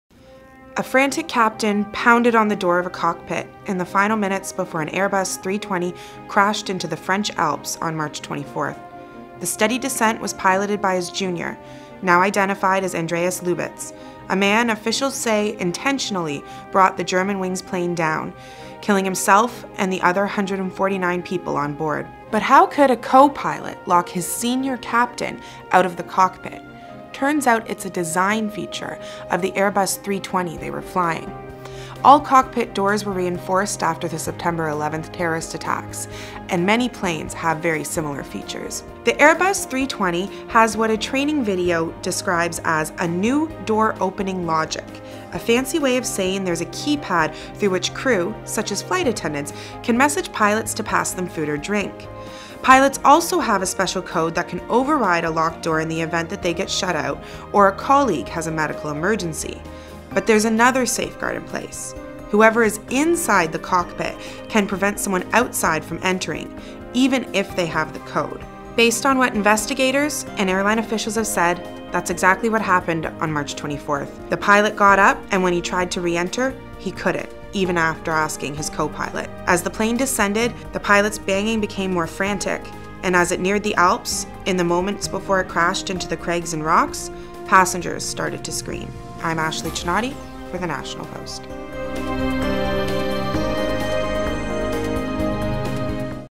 NP Explainer: How could a senior pilot get locked out of the cockpit?
A frantic captain pounded on the door of a cockpit, in the final minutes before an Airbus 320 crashed into the French Alps on March 24. But how could a co-pilot lock his senior captain out of the cockpit? The National Post’s Ashley Csanady explains.